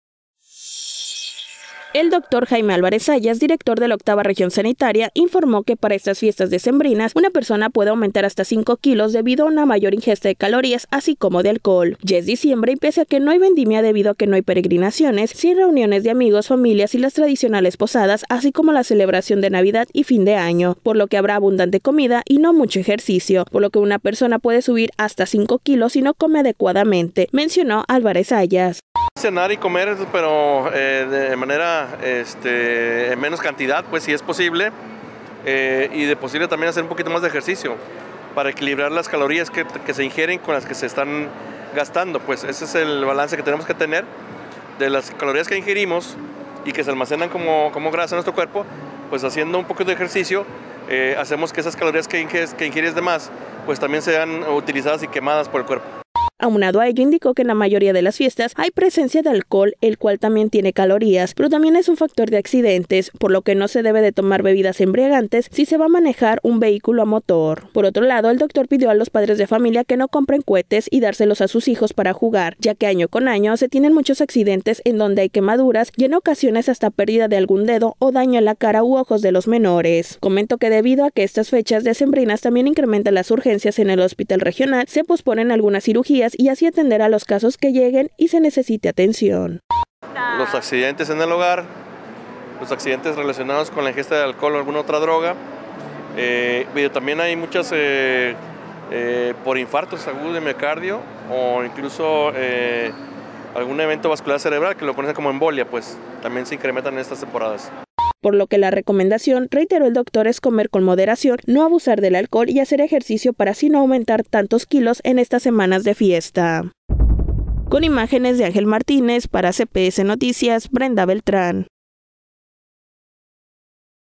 Hasta 5 kilos aumentan las personas por fiestas decembrinas | CPS Noticias Puerto Vallarta
Para estas fiestas decembrinas, una persona puede aumentar hasta cinco kilos debido a una mayor ingesta de calorías así como de alcohol.